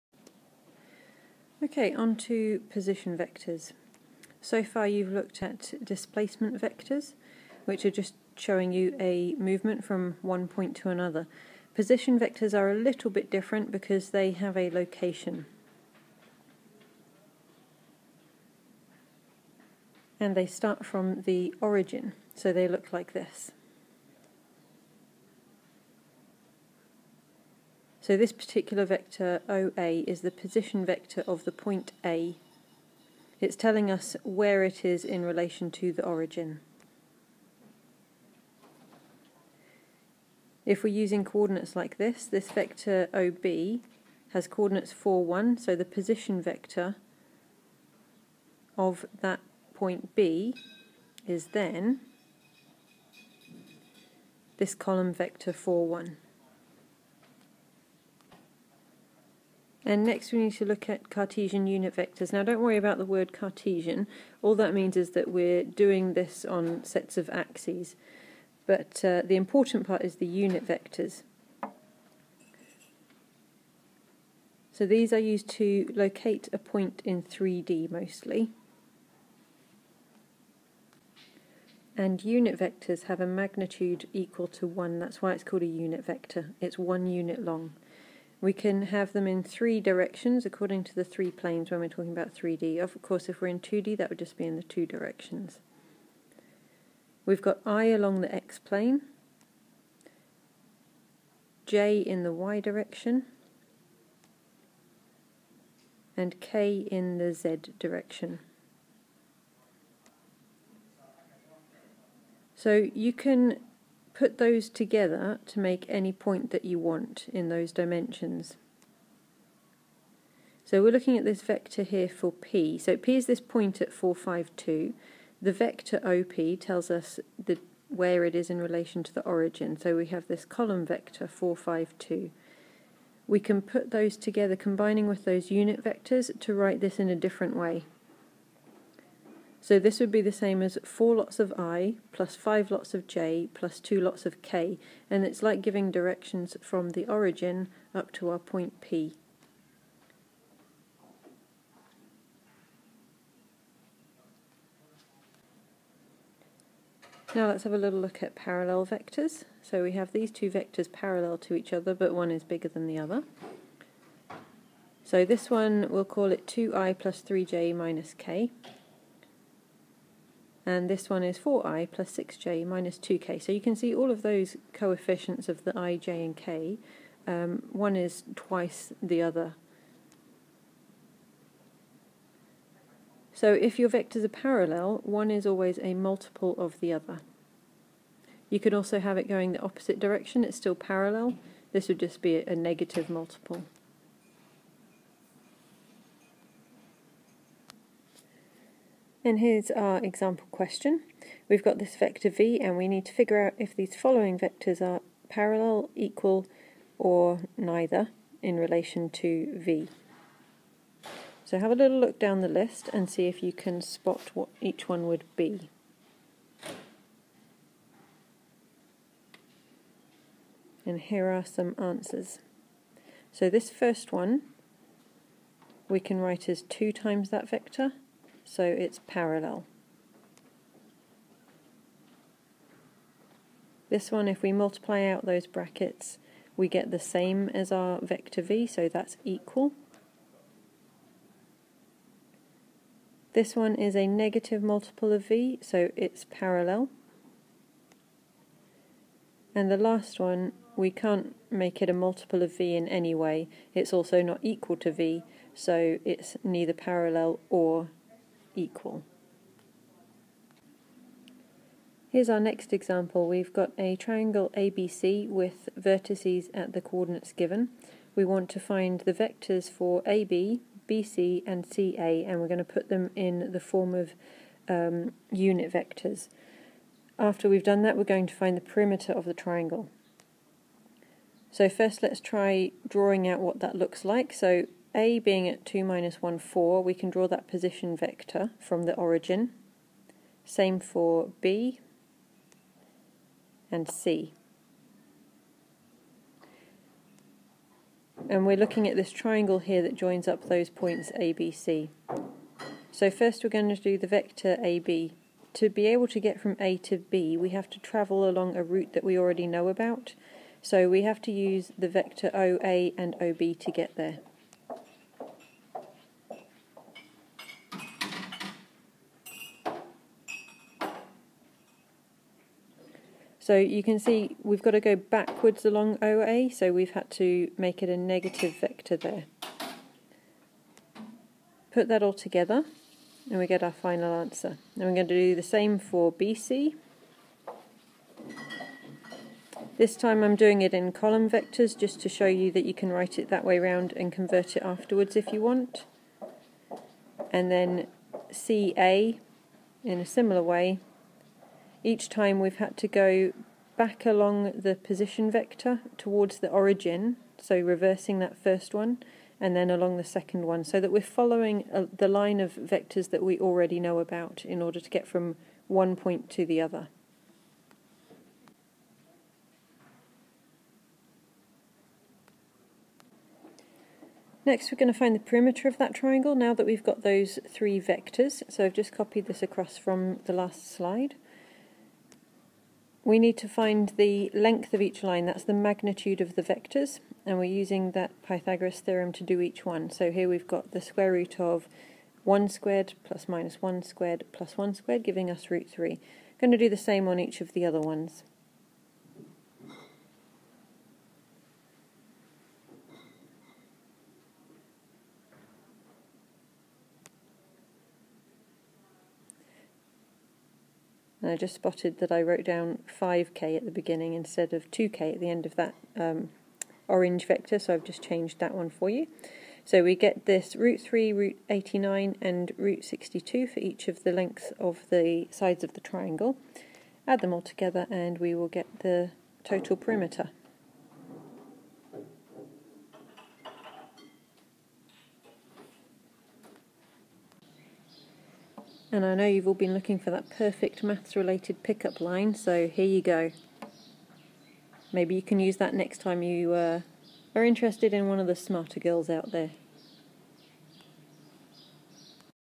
Vectors 1.2 Position, unit, parallel
Position vectors, unit vectors and parallel vectors. See the google docs sheet for the schedule of work for which questions should be done along side this video.